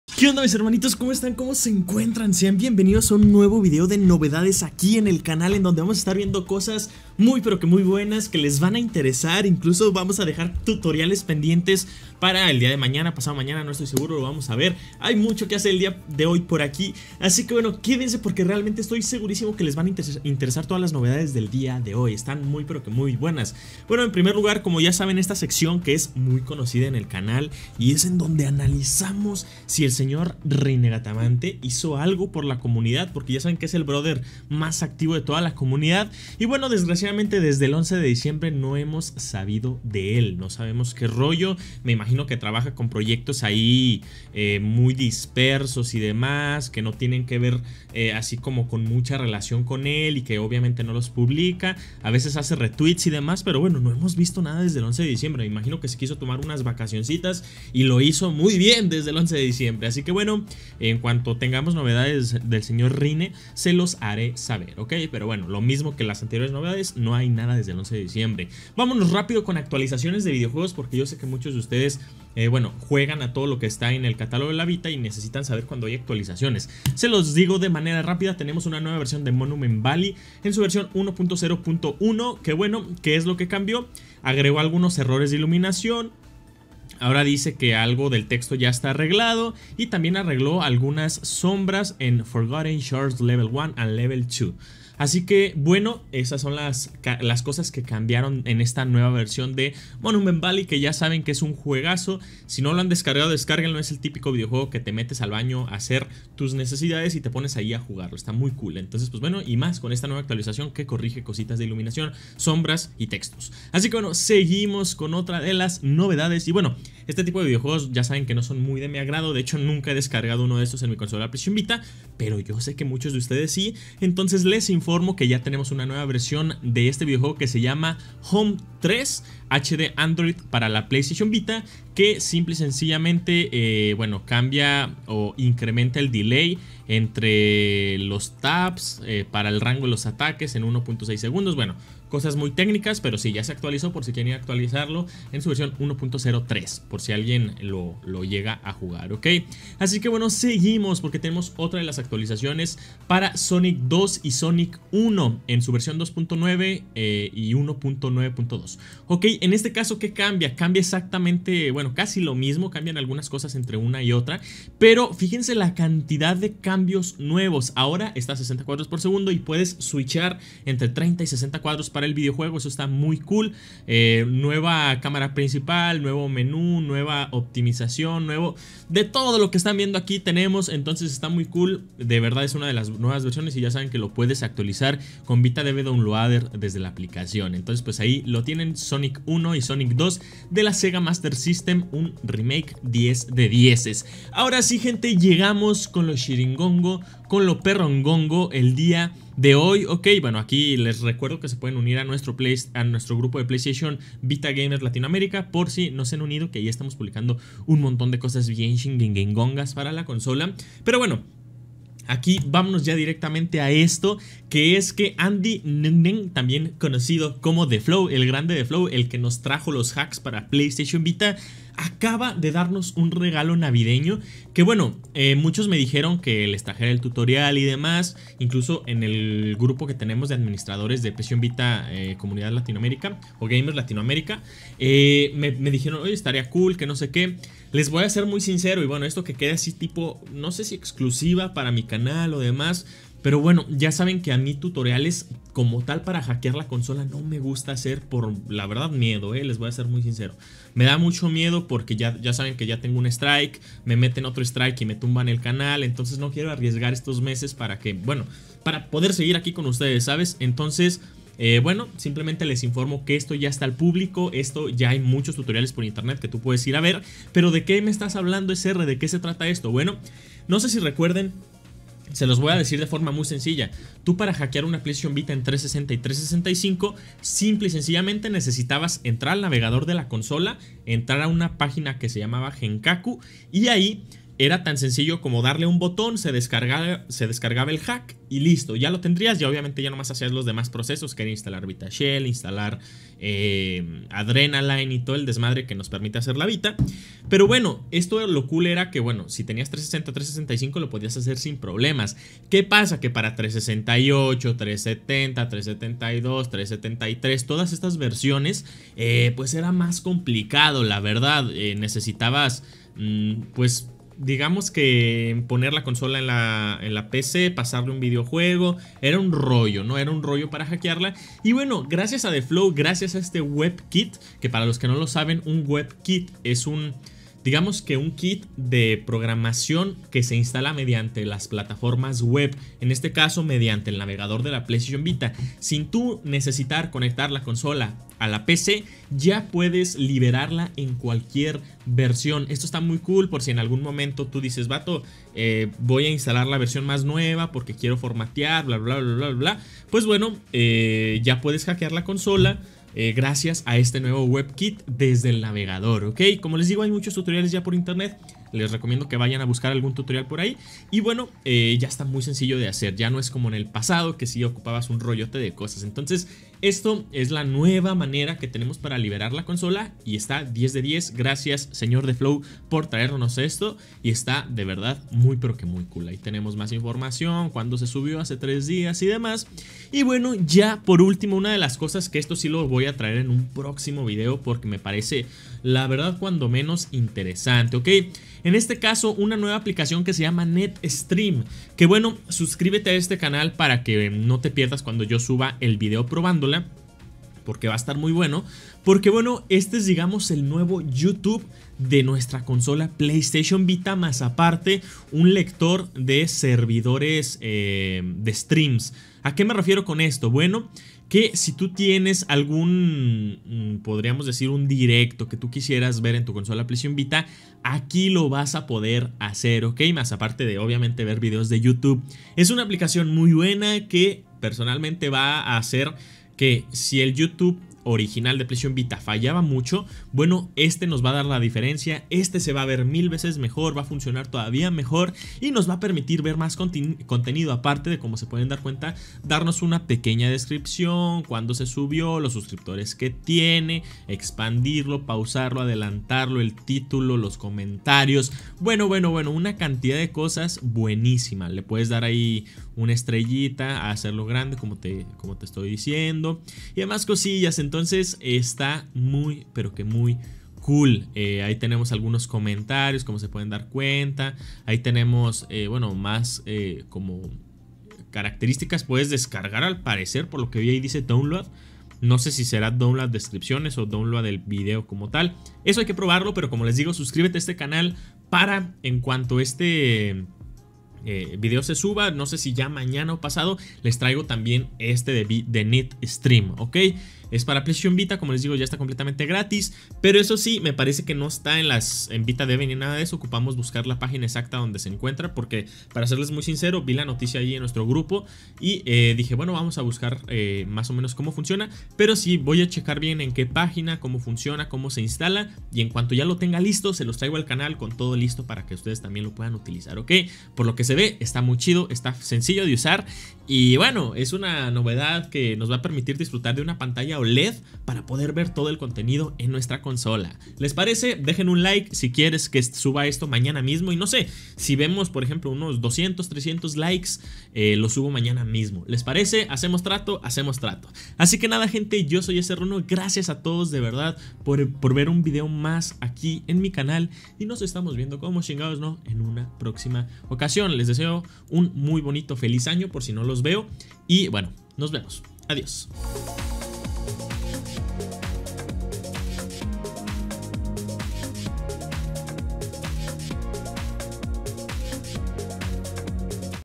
NOVEDADES PS VITA! Nuevo H4CK, Nuevo YT Vita, Nuevos UPDATES!
Hoy te mostraré TODO lo nuevo de PS VITA! Este será el ÚLTIMO video de novedades de Playstation Vita de 2022!

Canal de Rinne uno de los mejores desarrolladores de PS VITA!

¿Quieres apoyar a que tenga contenido de más calidad?